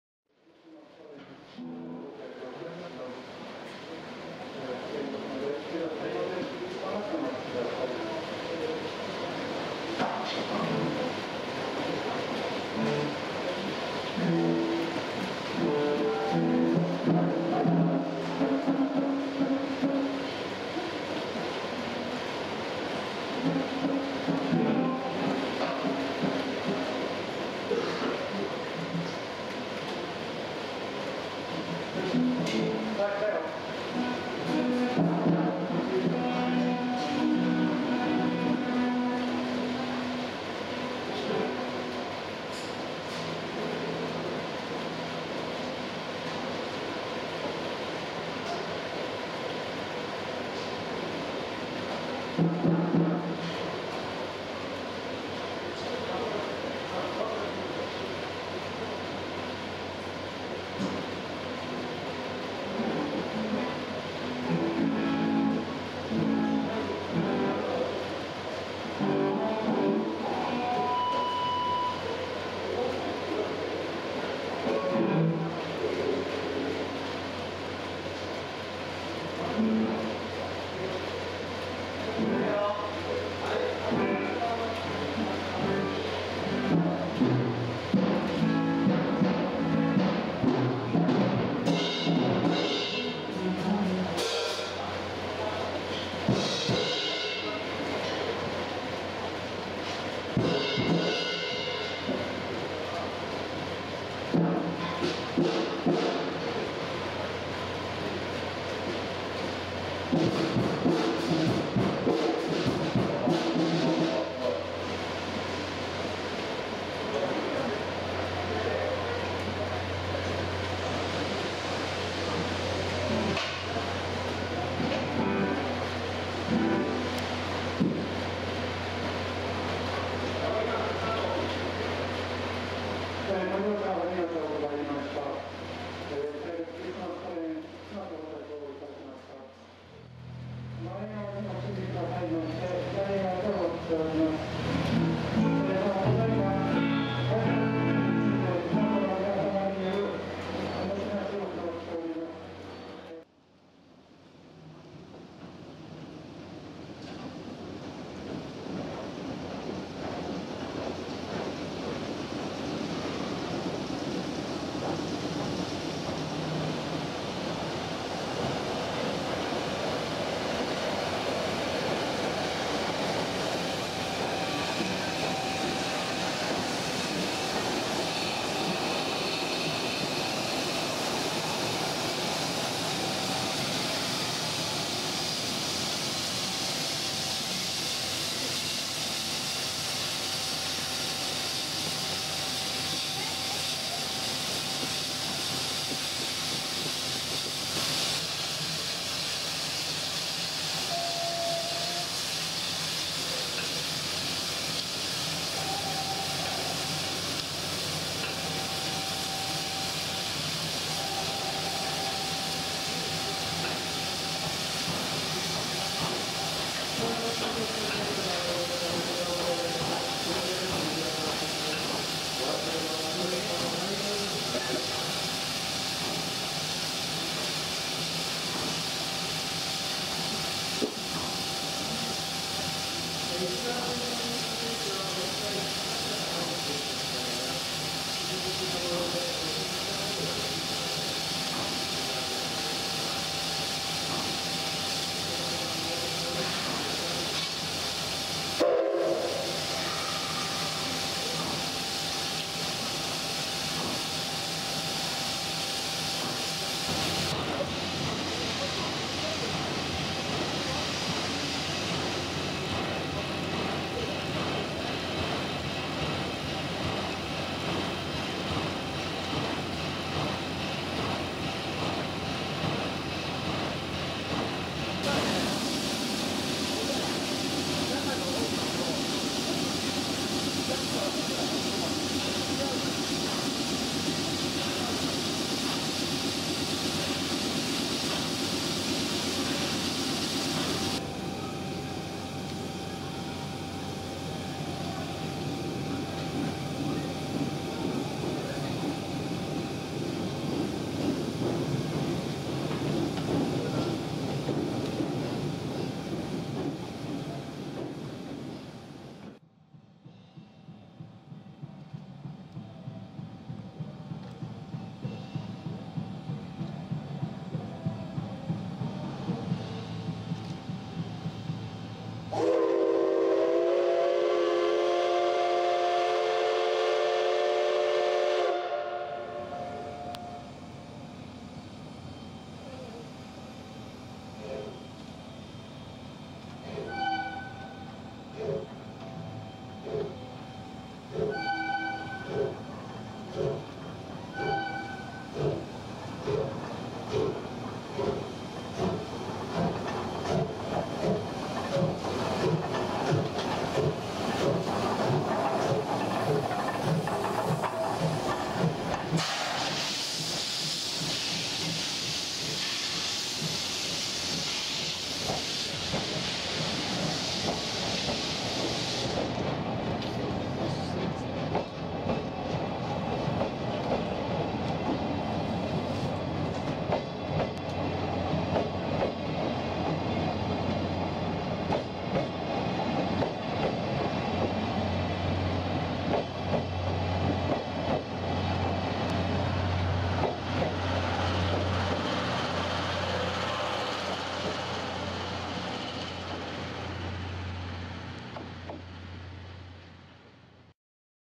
SLクリスマストレイン 会津若松駅 2015.12.05
2015年12月5日・6日　新潟～会津若松間で運転された
快速「SLクリスマストレイン」号。
会津若松駅到着の様子から、復路新潟行きの発車までの動画です。